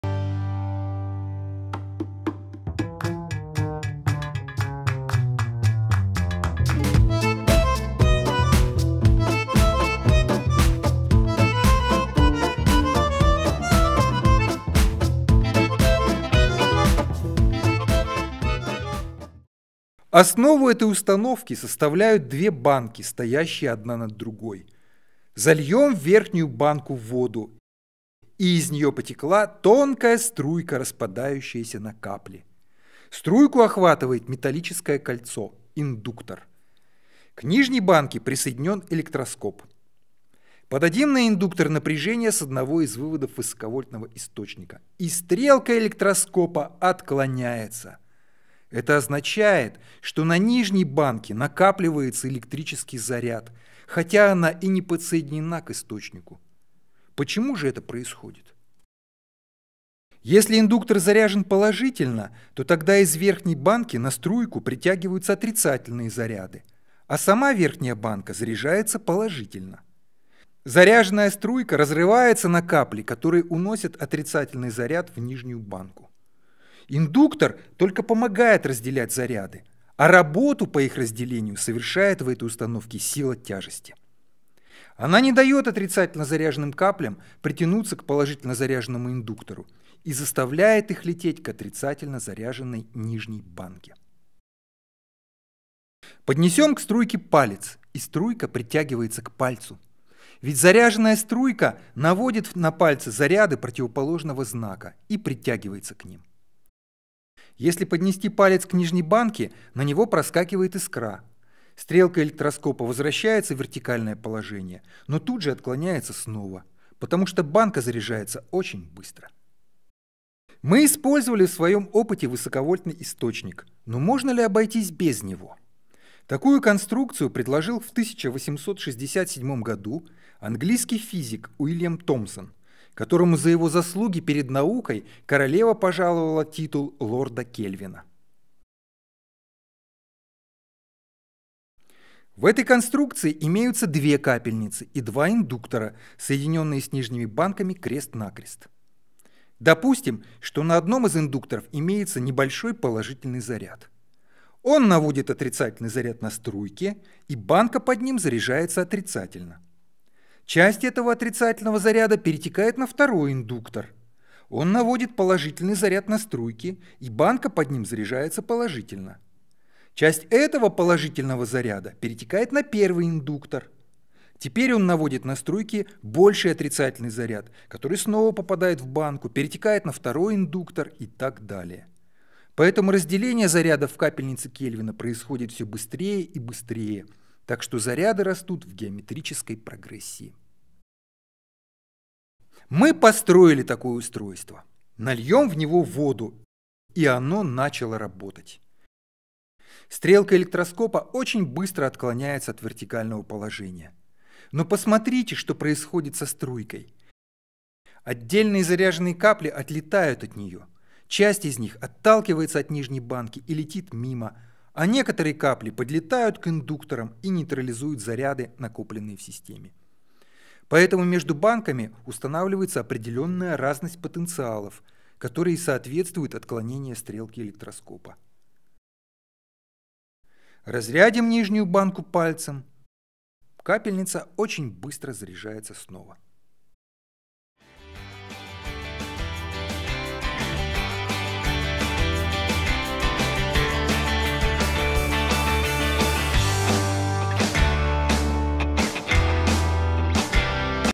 Капельница Кельвина
Капельница Кельвина — это электростатический генератор, работающий на индукционном принципе. Рабочим телом этого генератора служит вода, вытекающая двумя струйками через два перекрестных индуктора.